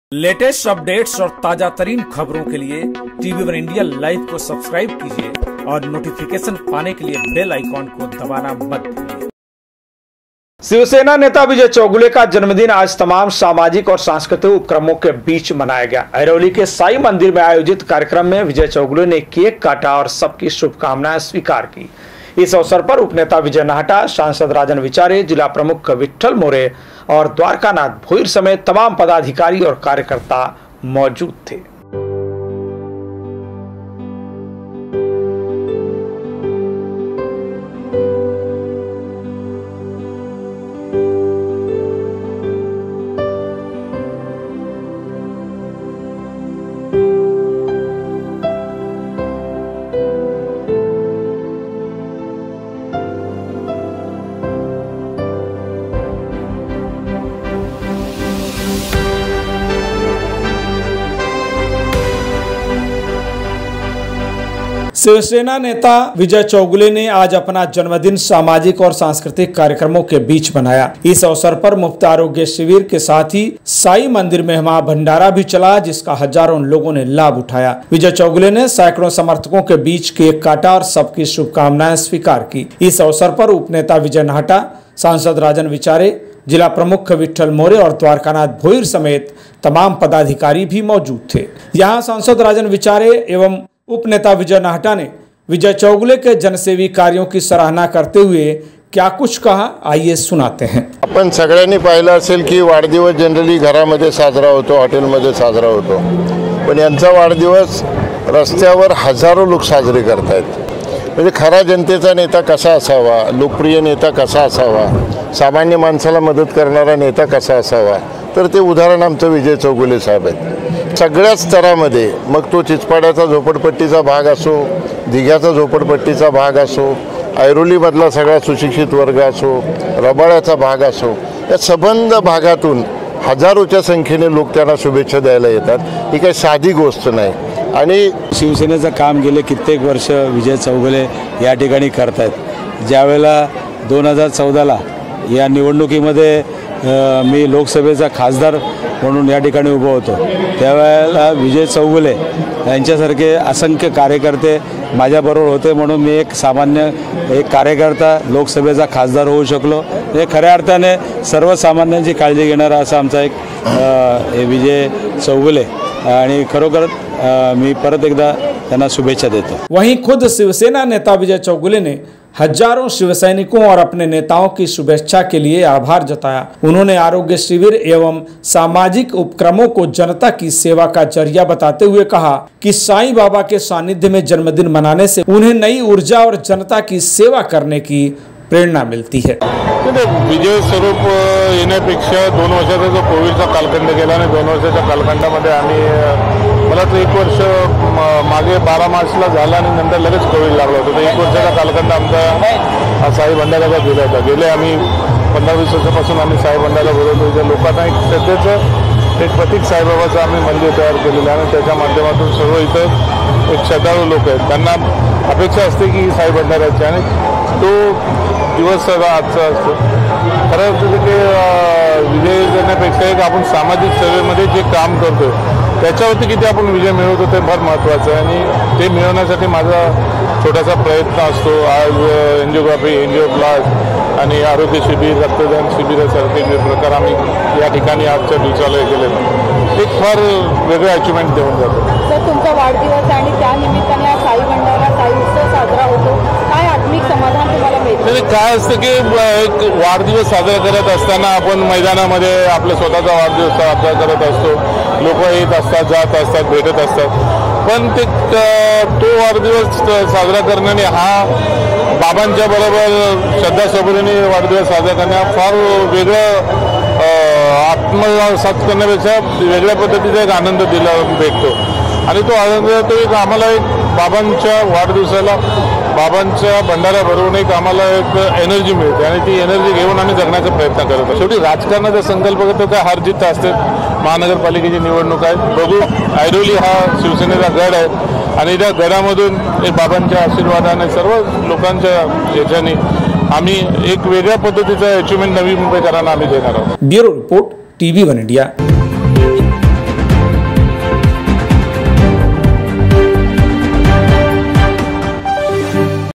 Vijay Chougule Birthday | साईं भंडारा और आरोग्य शिविर से साथ मना विजय चौगुले का जन्मदिन
शिवसेना नेता विजय चौगुले का जन्मदिन आज तमाम सामाजिक और सांस्कृतिक उपक्रमों के बीच मनाया गया. ऐरोली के साईंमंदिर में आयोजित कार्यक्रम में विजय चौगुले ने केक काटा और सबकी शुभकामनाएं स्वीकार की. इस अवसर पर उपनेता विजय नाहटा, सांसद राजन विचारे, जिलाप्रमुख विट्ठल मोरे, द्वारकानाथ भोईर समेत तमाम पदाधिकारी मौजूद थे.